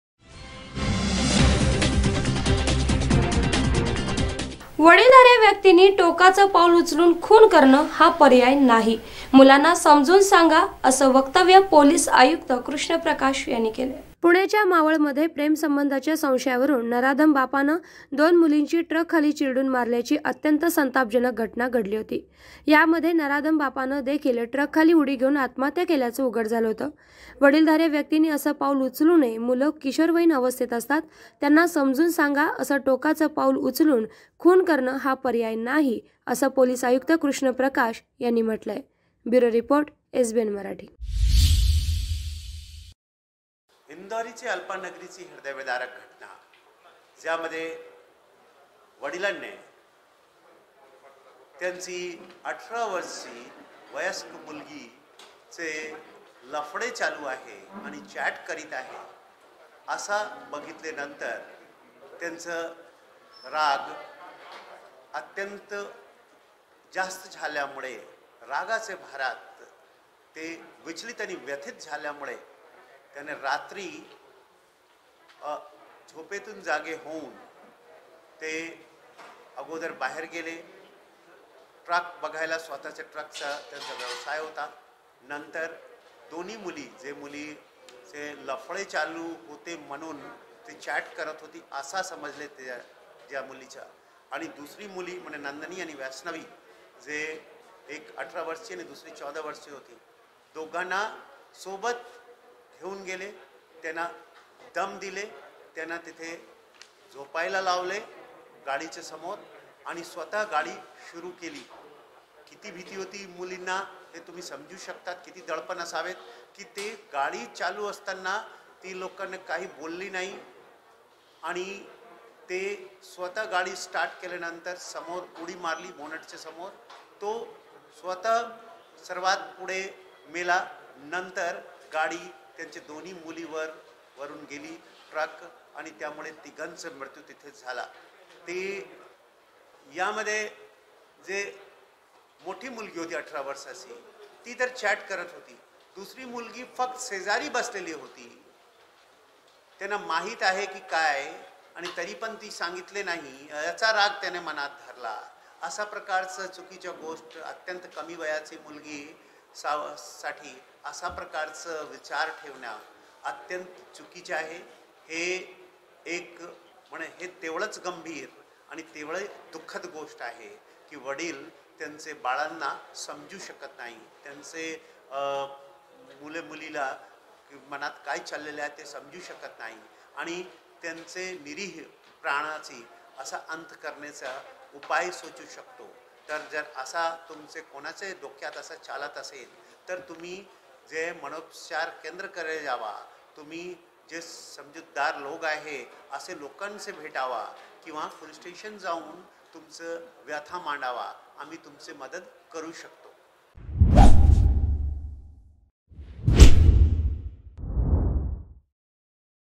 Pune | वडीलधाऱ्या व्यक्तींनी टोकाचं पाऊल उचलून खून करणे पर्याय नव्हे - पोलीस आयुक्त कृष्ण प्रकाश
वडीलधाऱ्या व्यक्तींनी टोकाचं पाऊल उचलून खून करणे पर्याय नव्हे - पोलीस आयुक्त कृष्ण प्रकाश.@